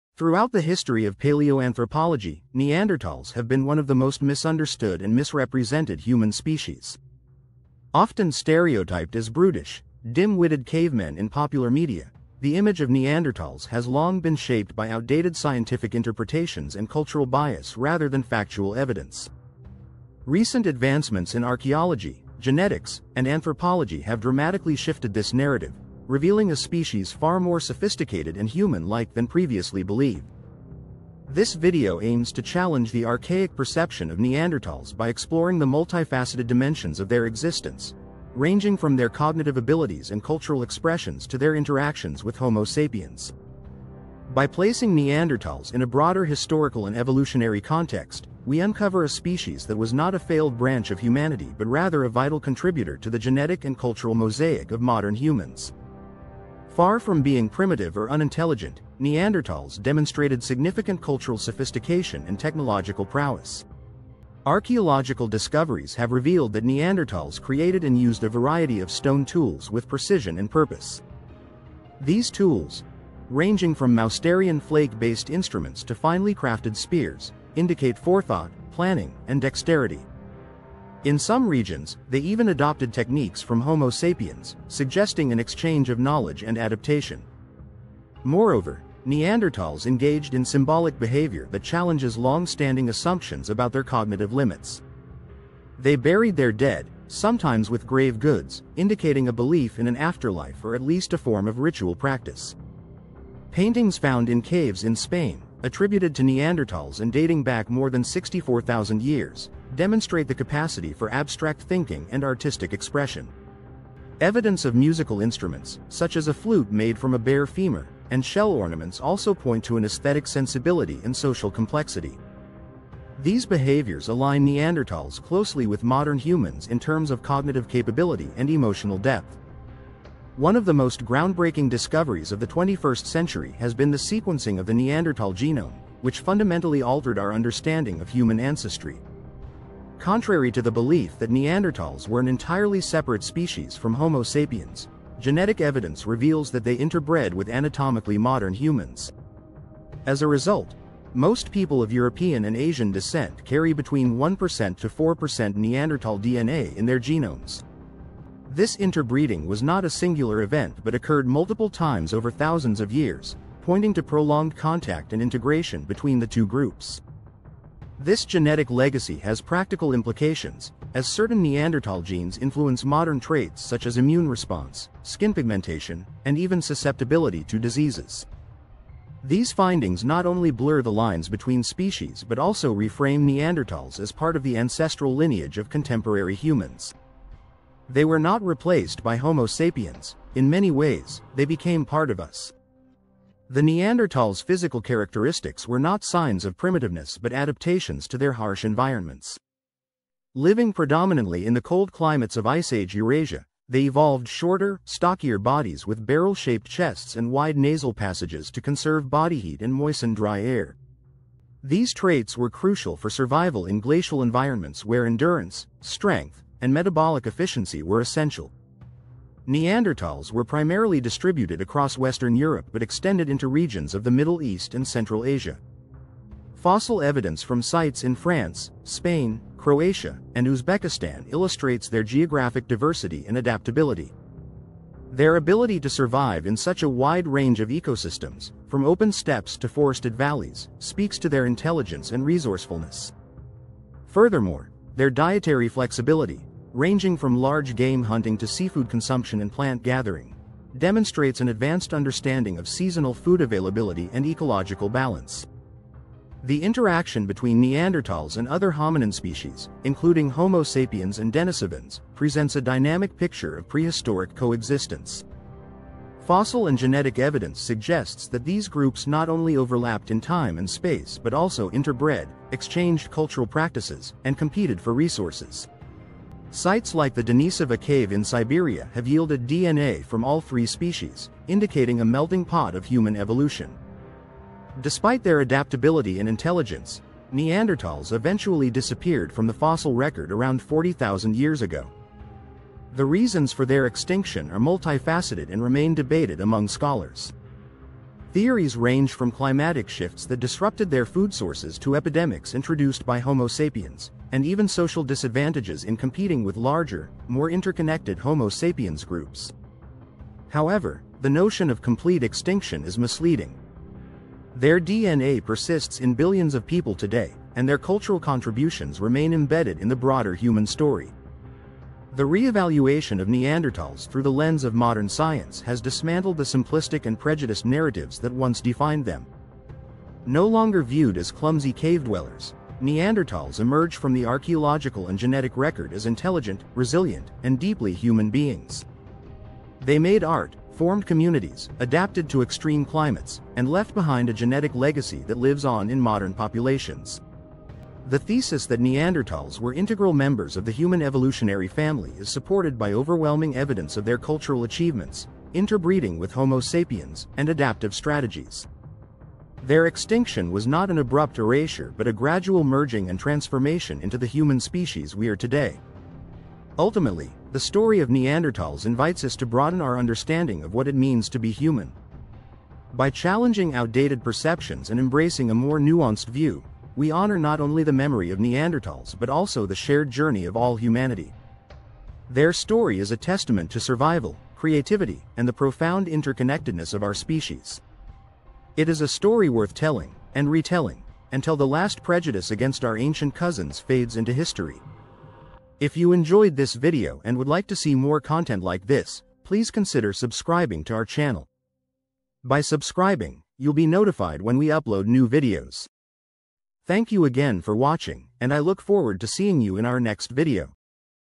Neanderthals Reimagined: The Truth About Our Ancient Human Cousins
Forget everything you thought you knew about Neanderthals. Once dismissed as primitive and brutish, recent scientific discoveries are rewriting their story entirely. In this documentary, we dive deep into the real lives of Neanderthals—exploring their intelligence, culture, art, and even their legacy in modern human DNA.

Using evidence from archaeology, genetics, and anthropology, this video reveals a species that buried their dead, created symbolic art, made complex tools, and interbred with Homo sapiens. From cave paintings to genome sequencing, the Neanderthals emerge not as evolutionary failures, but as vital contributors to the human story.

Join us as we uncover the truth behind the myths, and see how Neanderthals shaped the way we live, think, and even look today.